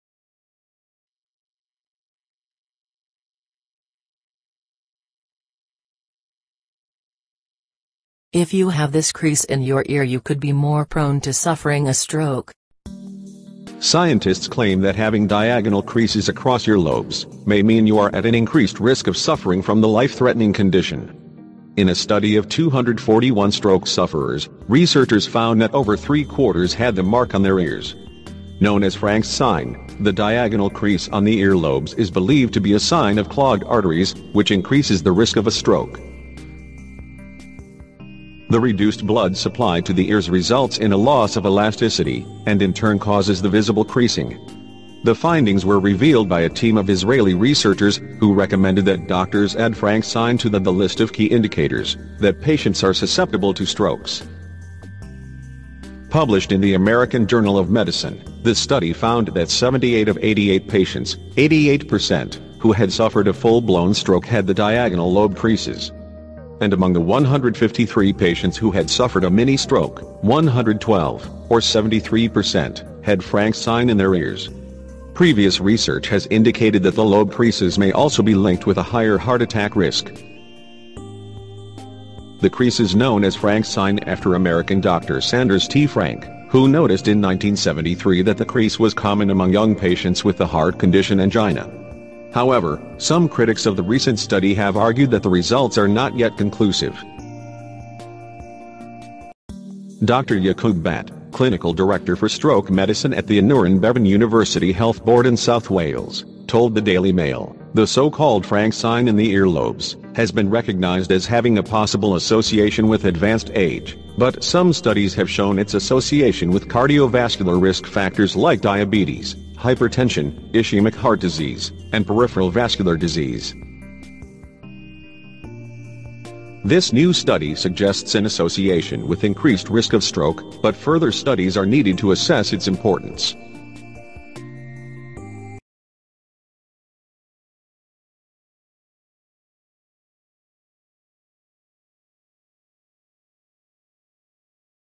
If you have this crease in your ear you could be more prone to suffering a stroke
Scientists claim that having diagonal creases across your lobes may mean you are at an increased risk of suffering from a stroke.